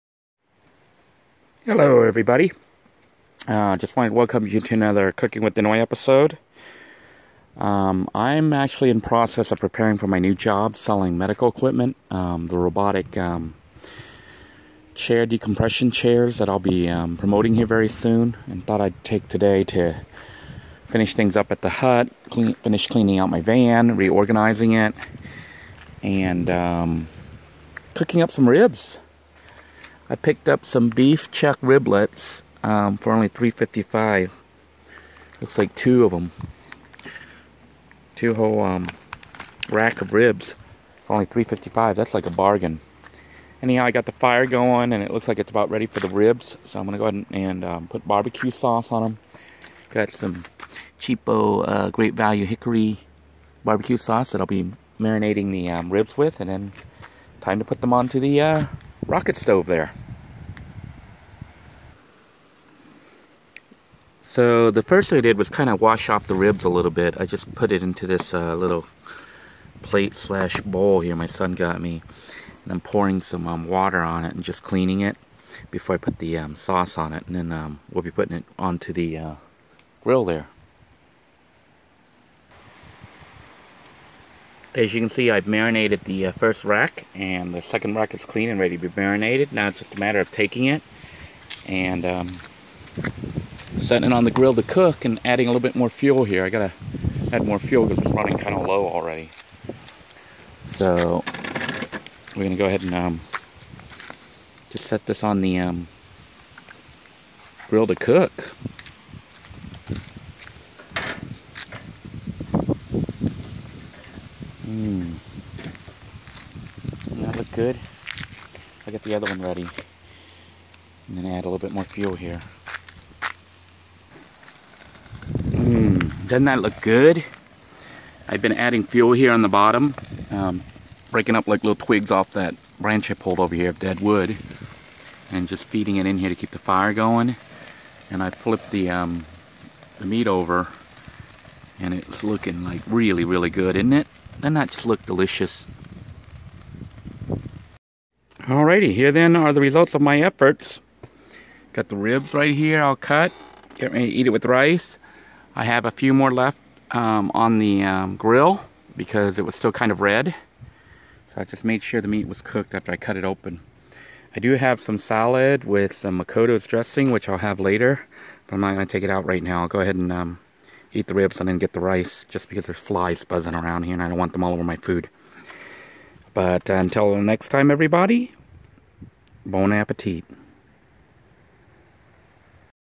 Vanlife - Cooking Cheap But Delicious Ribs On A Rocket Stove (V410) Minimalist Living In A Van
BBQ ribs barbecue on a rocket stove very nicely. The barbecue ribs taste great. Fantastic BBQ ribs.  I love BBQ ribs! It turns out the rocket stove I created from the old mailbox is actually pretty useful for cooking up some delicious meals at The Hut.